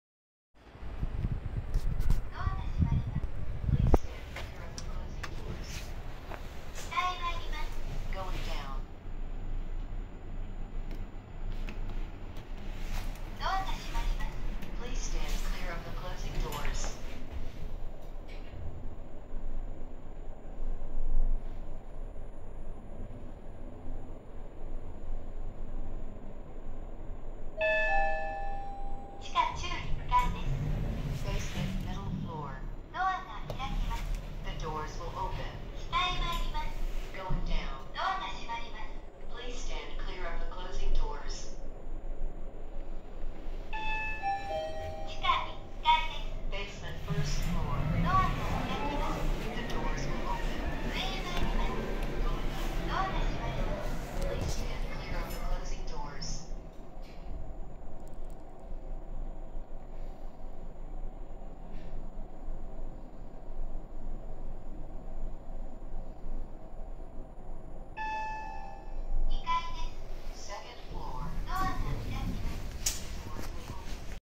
麻布台ヒルズのエレベーター（ガーデンプラザ・A）
メーカーはオーチス製です。2023年11月24日に開業した麻布台ヒルズです。全体的にかご内はレトロ感があります。

用途：乗用
定員：20名
積載：1300kg
設置年：2023年
アナウンス：あり
到着音：あり
車椅子操作盤：あり
エレベーター台数：1
機種：オーダーレボ
方式：ロープ式